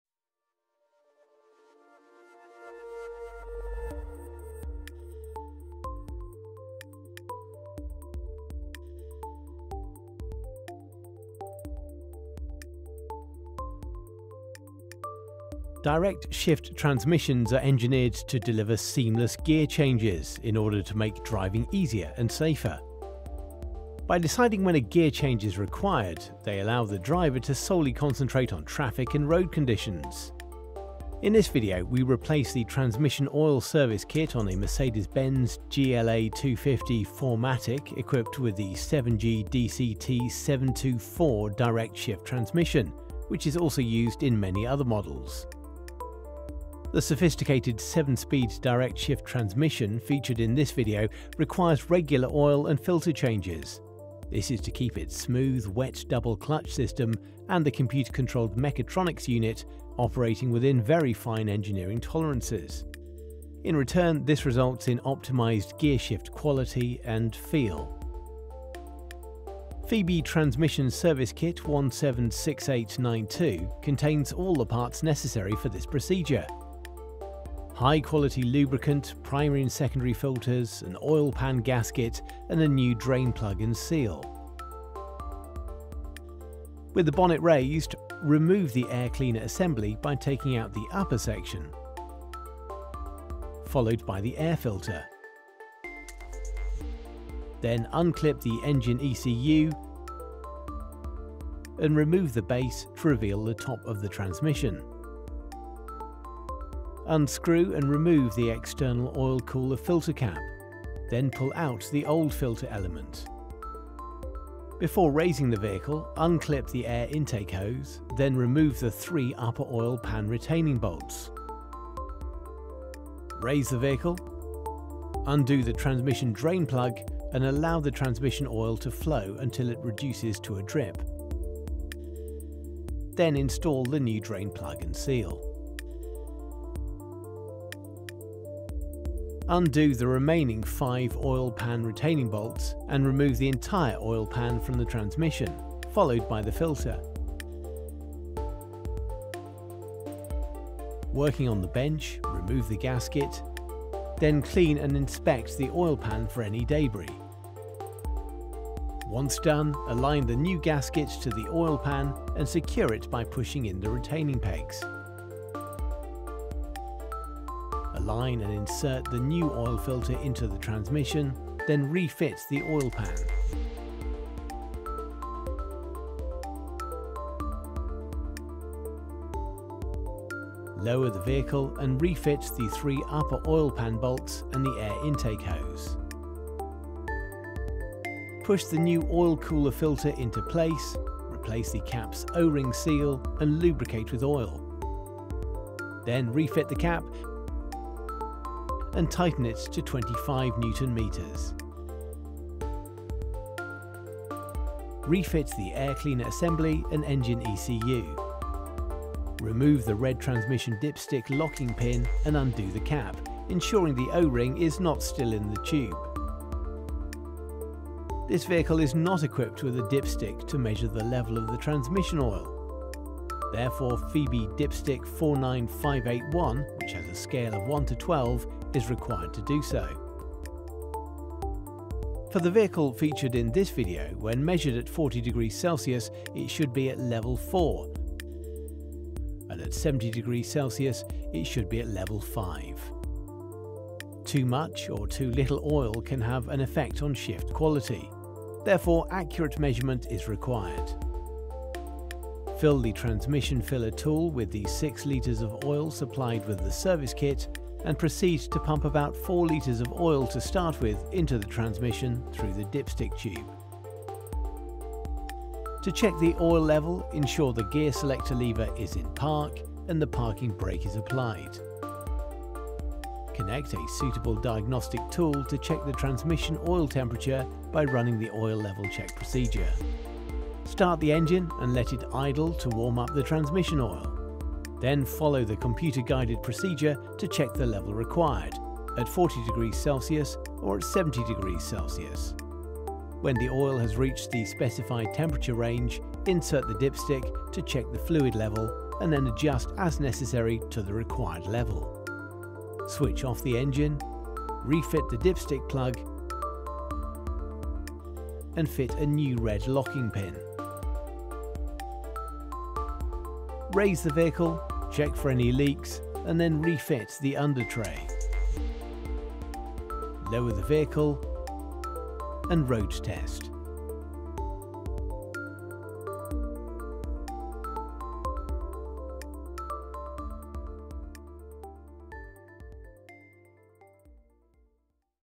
[EN] Mercedes 7G DCT 724 Automatic Transmission Oil and Filter Replacement
In this video, we replace the Transmission Oil Service Kit on a Mercedes-Benz GLA 250 4-Matic equipped with the 7G DCT 724 direct-shift transmission - which is also used in many other models.
The sophisticated 7-speed direct-shift transmission featured in this video requires regular oil and filter changes. This is to keep its smooth, wet double-clutch system - and the computer-controlled mechatronics unit - operating within very fine engineering tolerances. In turn, this results in optimised gearshift quality and ‘feel’.
febi Transmission Service Kit 176892 contains all the parts necessary for this procedure: high-quality lubricant, primary and secondary filters, an oil pan gasket, and a new drain plug and seal.